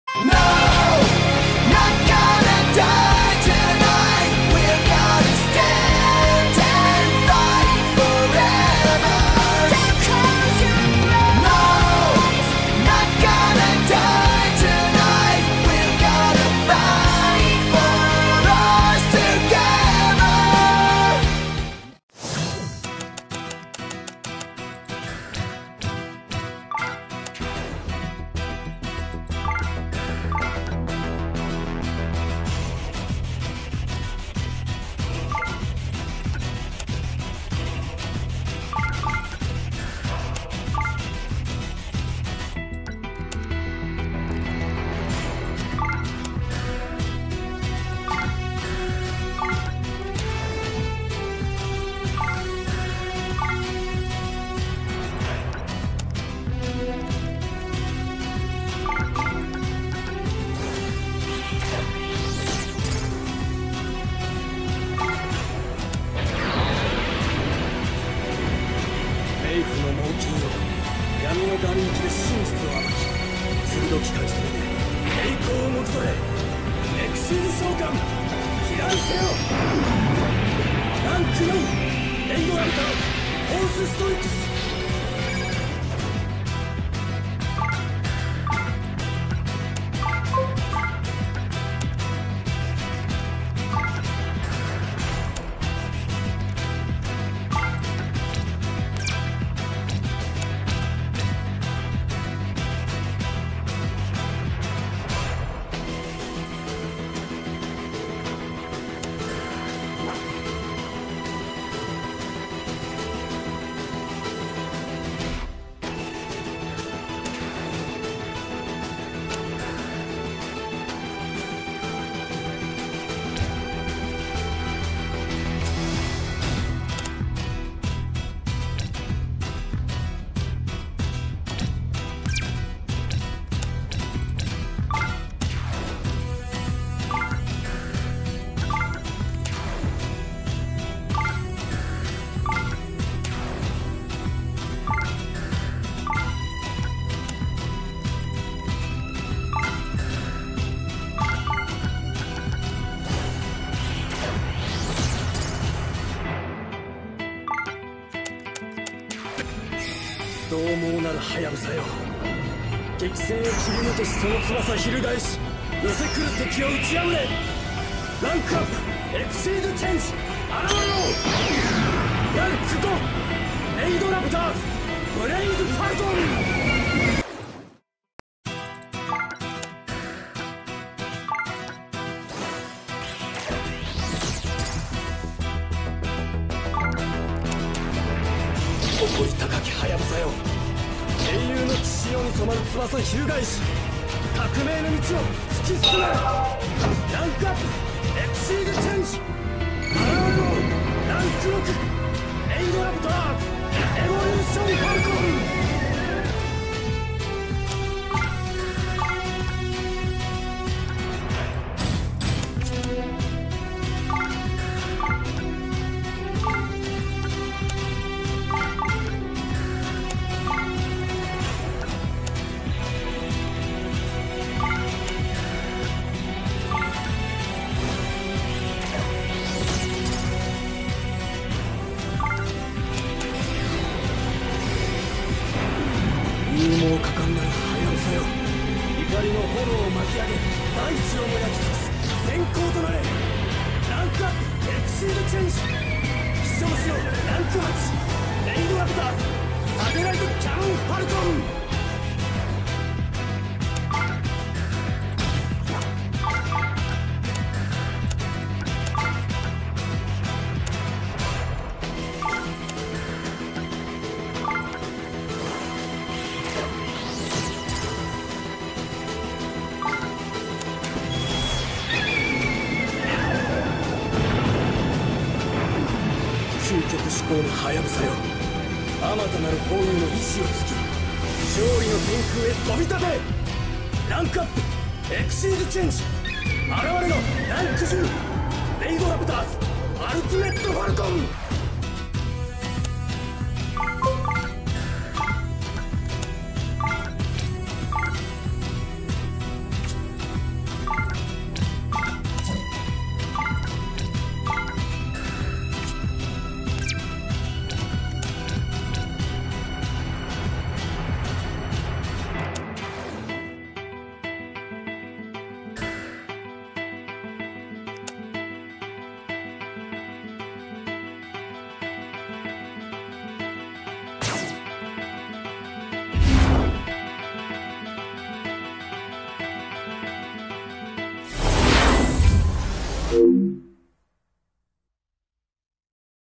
rank-up raidraptor force strix - Ultimate Falcon
One turn rank-up from raidraptor blaze falcon to ultimate falcon.Anime cinematix.